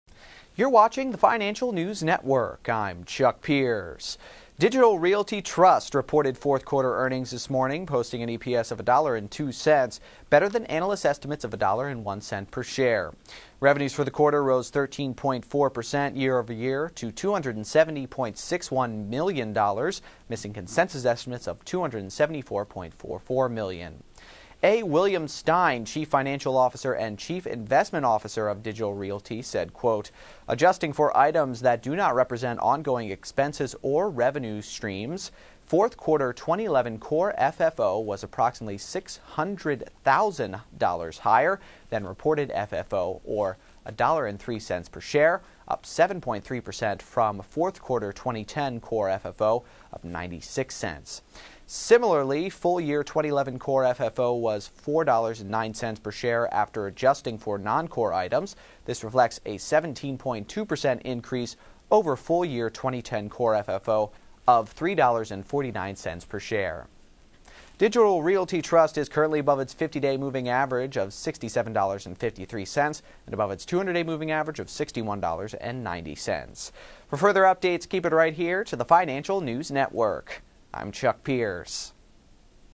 Digital Realty Trust Reports Mixed Estimates For Q4, Rose 13.4% Year-Over-Year
Digital Realty Trust (NYSE:DLR) reported Q4 EPS of $1.02, better than analyst estimates of $1.01 per share. Revenues for the quarter rose 13.4% year-over-year to $270.61 million, missing consensus estimates of $274.44 million.

A. William Stein, Chief Financial Officer and Chief Investment Officer of Digital Realty said, "Adjusting for items that do not represent ongoing expenses or revenues streams, fourth quarter 2011 core FFO was approximately $600,000 higher than reported FFO, or $1.03 per share, up 7.3% from fourth quarter 2010 core FFO of $0.96.  Similarly, full year 2011 core FFO was $4.09 per share after adjusting for non-core items. This reflects a 17.2% increase over full year 2010 core FFO of $3.49 per share."

Digital Realty Trust is currently above its 50-day moving average (MA) of $67.53 and above its 200-day of $61.90.